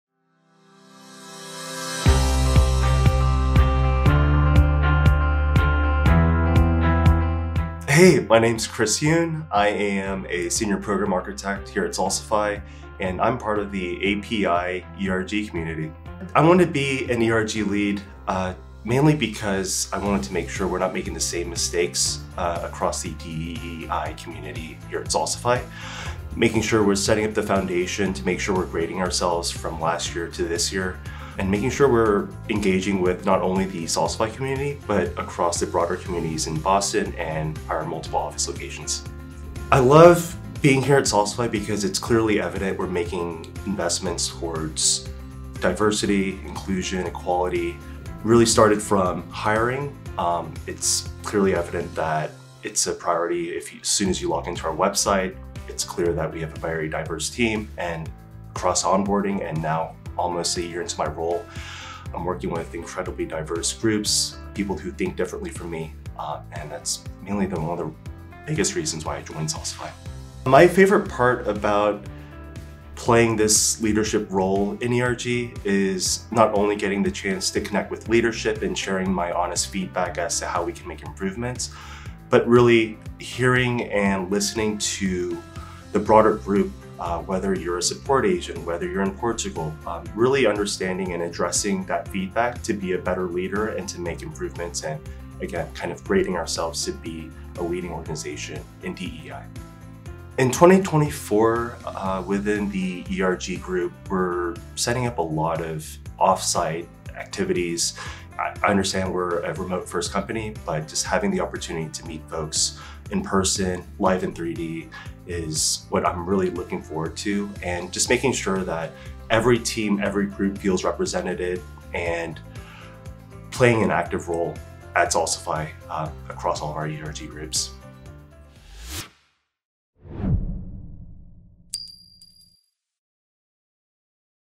Enhancing ERG Initiatives at Salsify
Chris Yun, Senior Program Architect at Salsify and ERG lead, as he shares his insights and experiences in driving equity, diversity, and inclusion (ED&I) within the company. In this video, Chris discusses the importance of setting a strong foundation for ED&I, engaging with the broader community, and ensuring continuous improvement.

Download our equity, diversity, and inclusion (ED&I) report to learn about our commitment to fostering a safe and equitable culture.